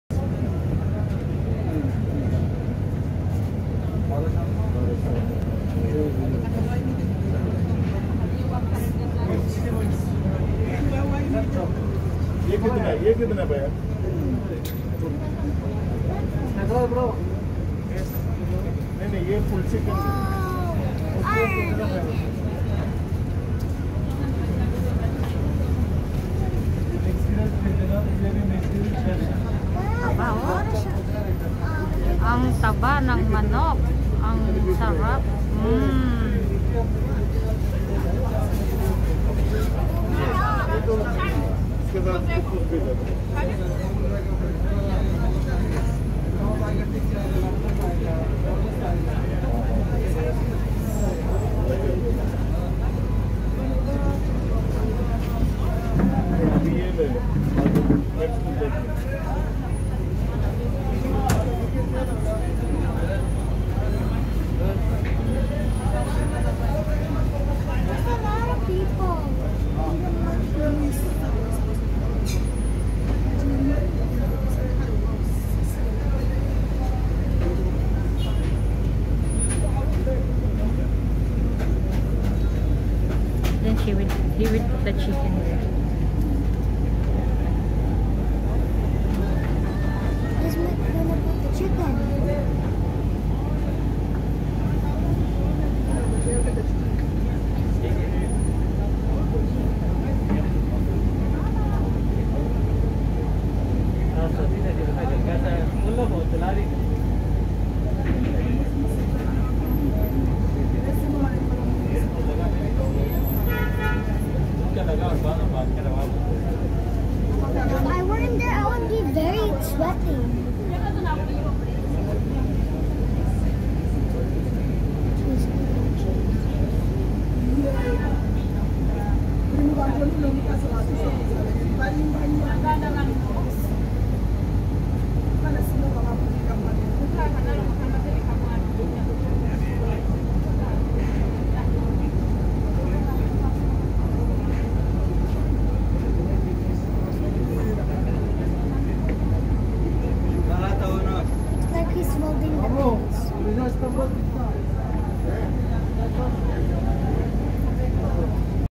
Dinner for tonight|Authentic Arabic Shawarma
Arabic shawarma chicken broast are the best offer in this place@Sameeh Restaurant..
Yummy foodie dinner..